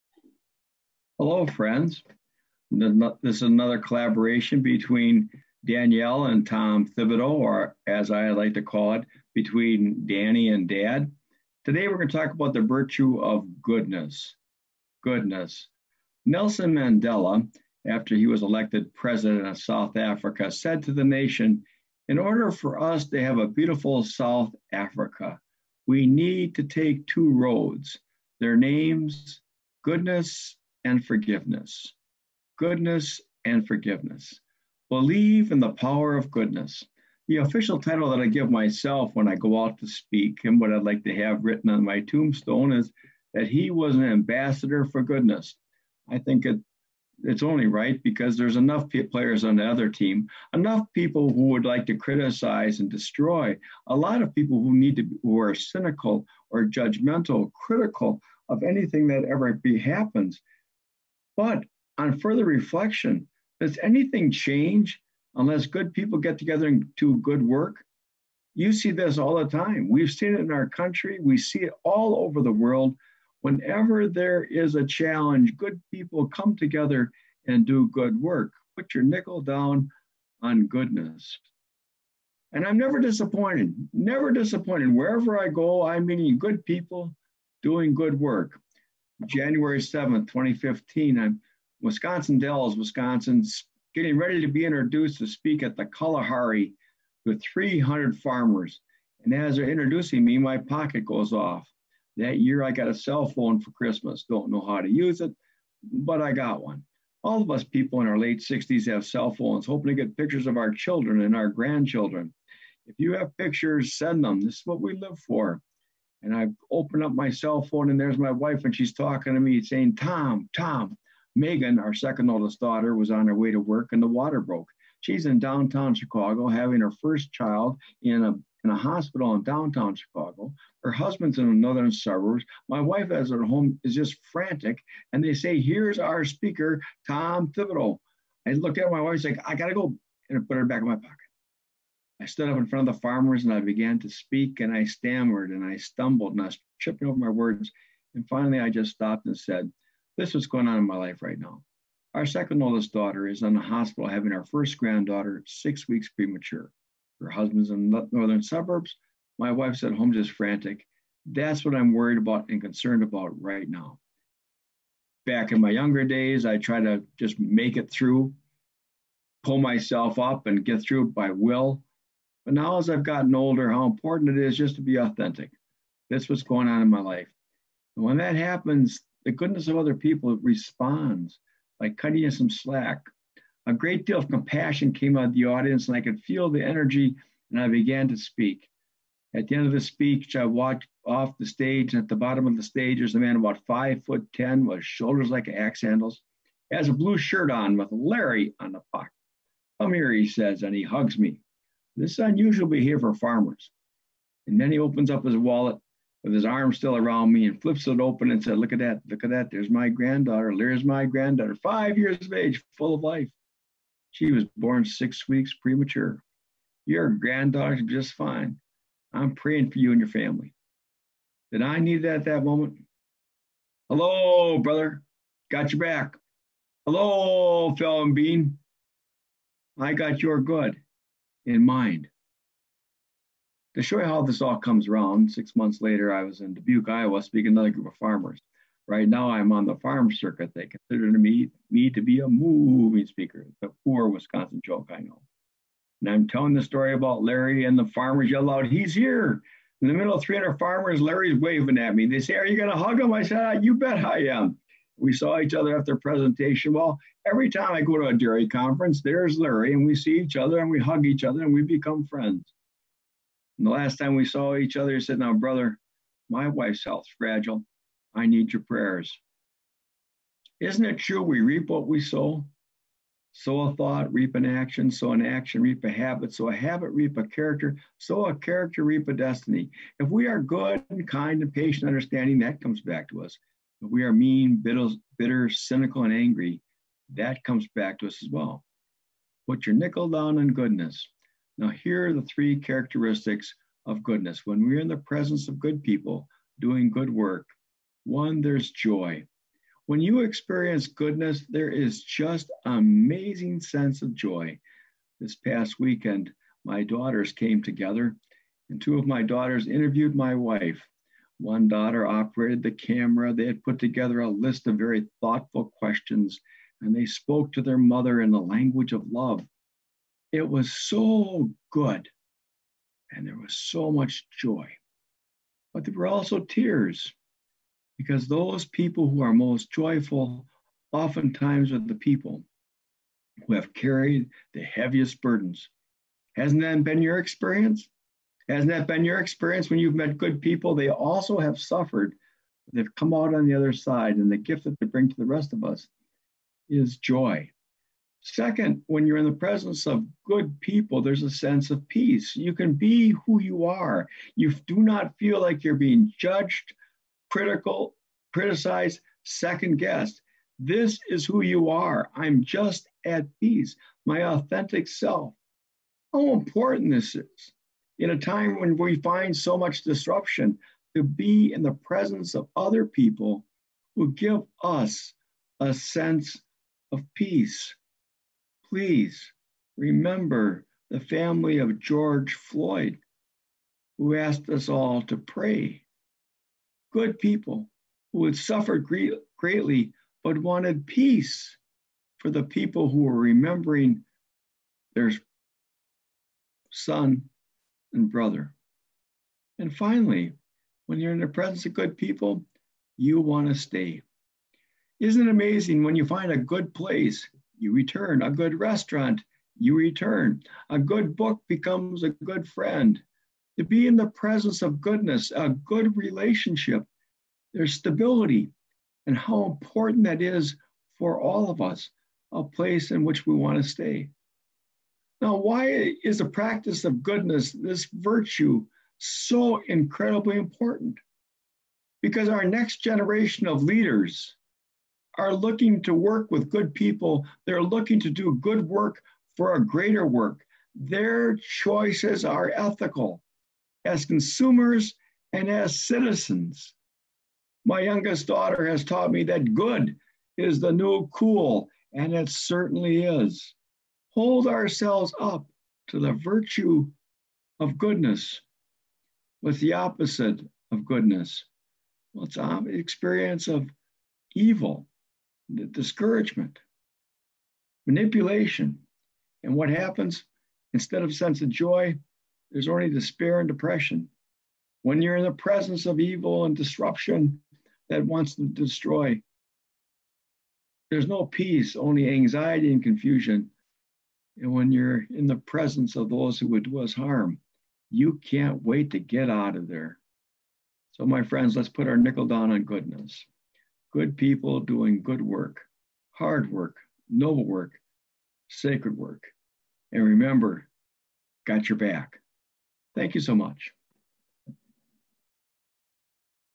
Be An Ambassador for Goodness
The world needs more goodness. How you can be an ambassador for goodness through servant leadership.